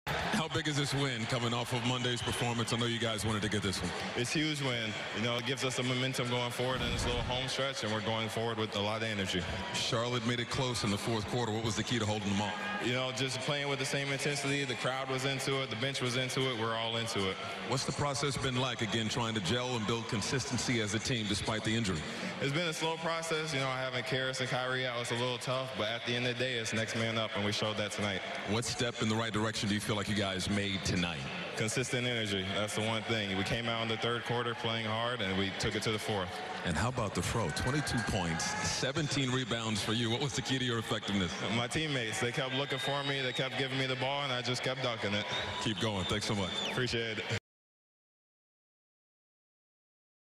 Jarrett Allen has monster 22-point, 17-rebound game vs. Hornets
Nets big man Jarrett Allen enjoyed one of the best games of his young career Wednesday night against the Hornets, scoring a game-high 22 points and pulling down 17 boards... and he's still just 21 years old.